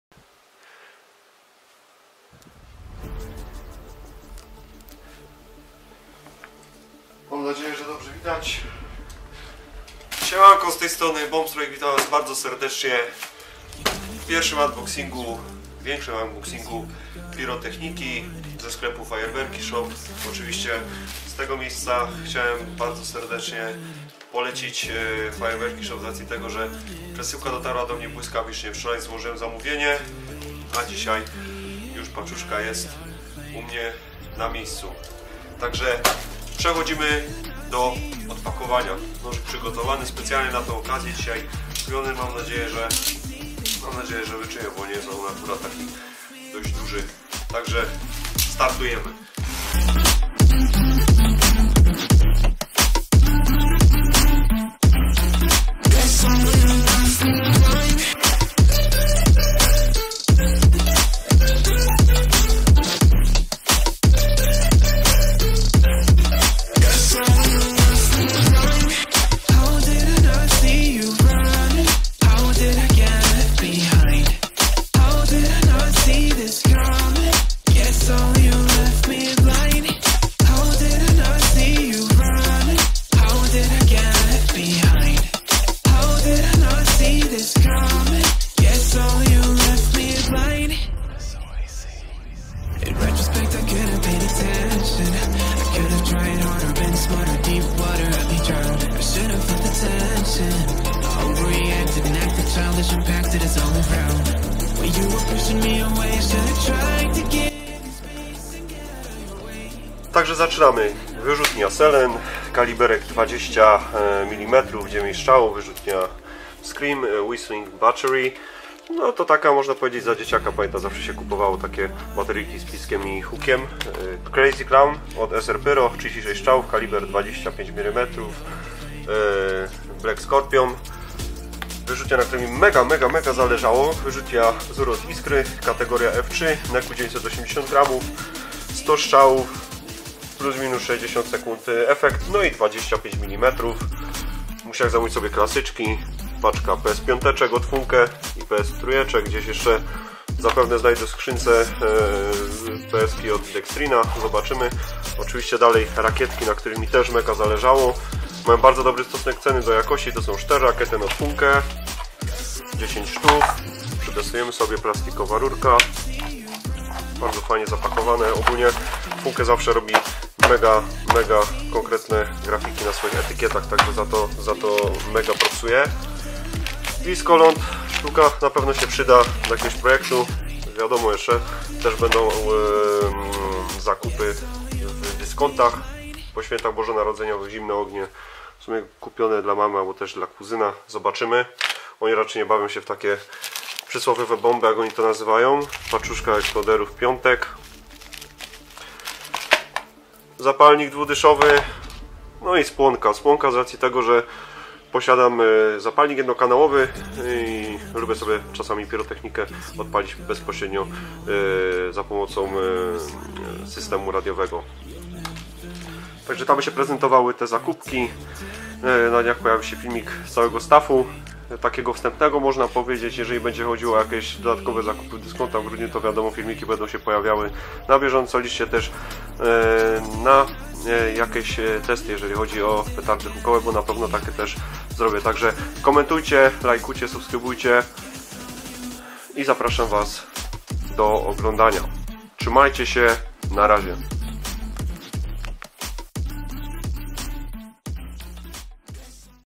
Unboxing pirotechniki ze sklepu FAJERWERKI_SHOP 🎆
Powoli szykujemy się do Sylwestra 🎆

ZOSATWCIE SUBA, KOMENTARZ ALBO ŁAPKĘ W GÓRĘ! 🎆